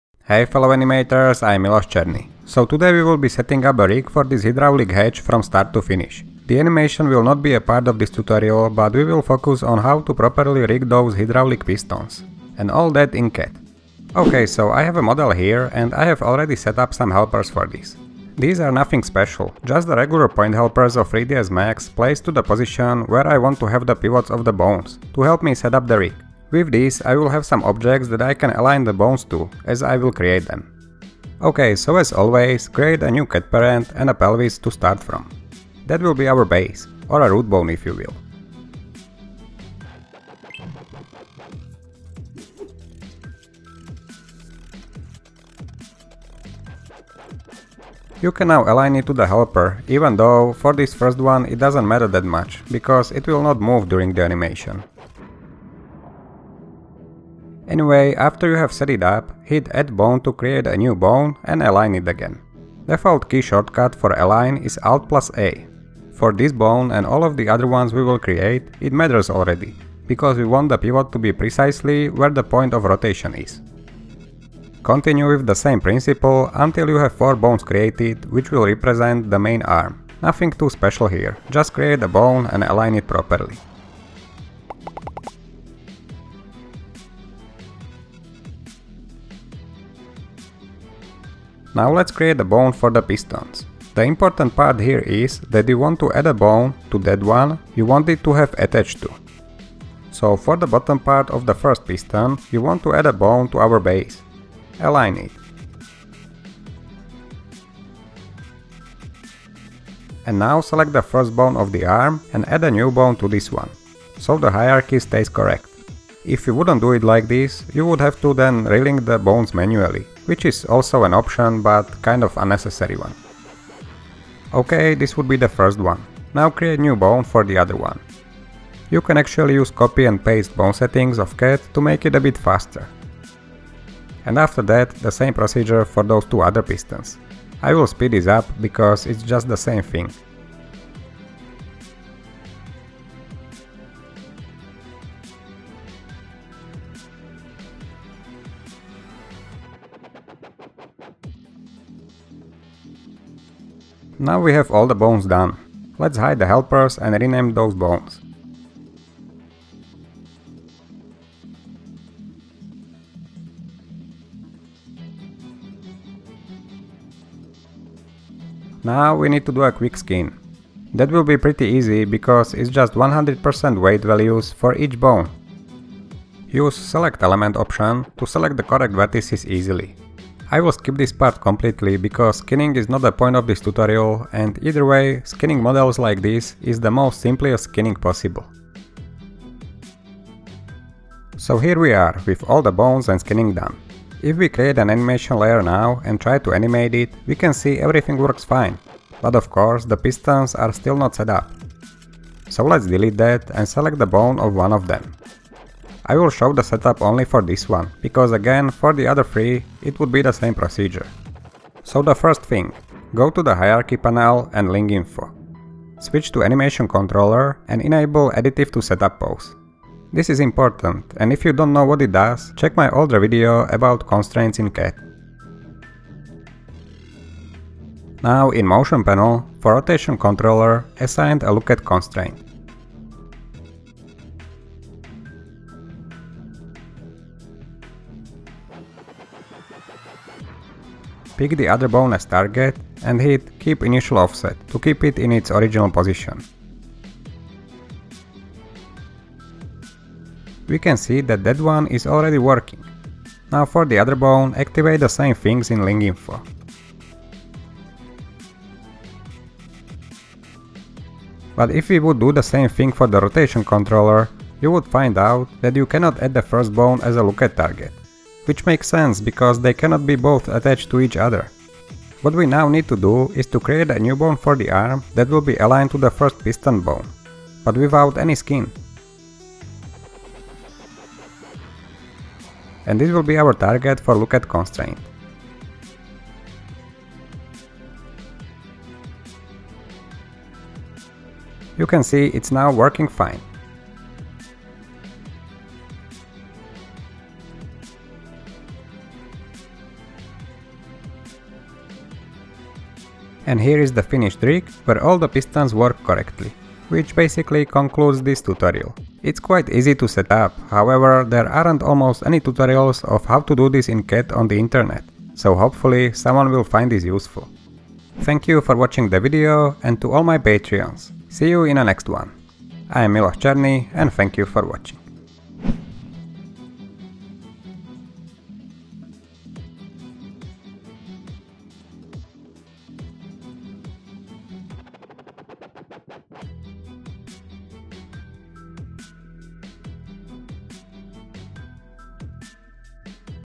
Hydraulic Pistons Mechanical Rig - 3ds Max CAT Rigging Tutorial
Here is a new tutorial. Today about rigging hydraulic pistons in CAT. There aren't many information about this around the internet, so hopefully you will find it interesting :)

If you want to learn more about CAT (Character Animation Toolkit) check some of my other videos. This whole channel is mainly dedicated to CAT animation system.

If you would like to add subtitles for your own language, feel free to do so: